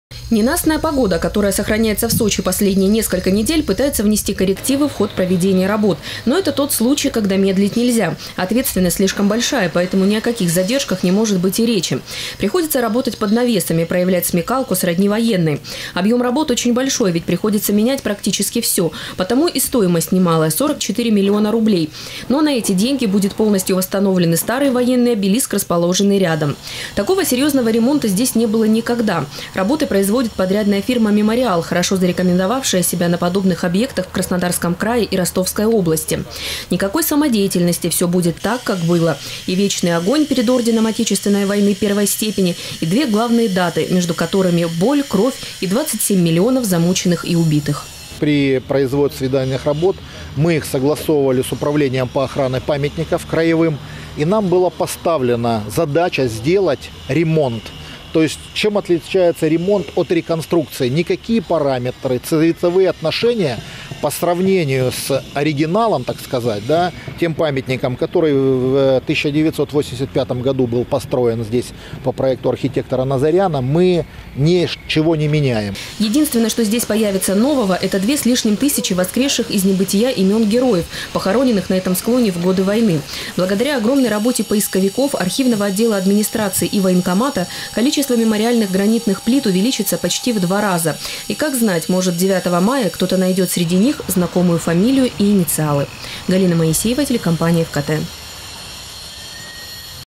Мемориал в Завокзальном районе Сочи возродиться 1 мая. Новости 24 Сочи Эфкате
Сейчас там полным ходом идут ремонтные работы. По словам подрядчиков, несмотря на определенные трудности, объект будет сдан точно в срок.
Сочи. Новости.
Телеканал Эфкате.